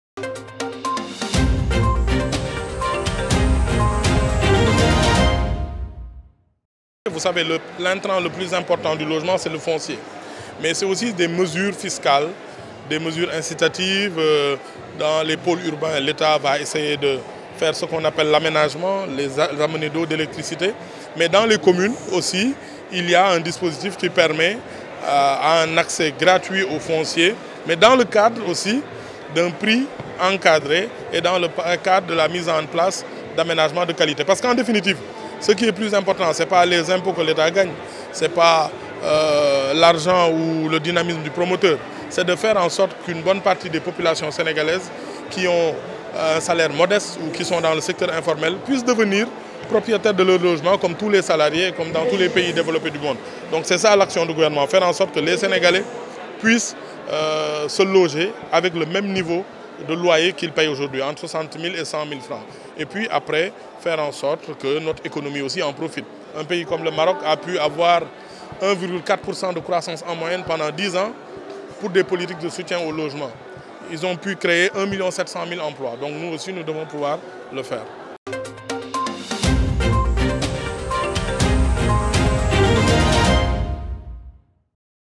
A. Karim Fofana : « Faire en sorte que les populations deviennent propriétaires… »
Abdou Karim Fofana : « Faire en sorte que les populations deviennent propriétaires de leurs logements… »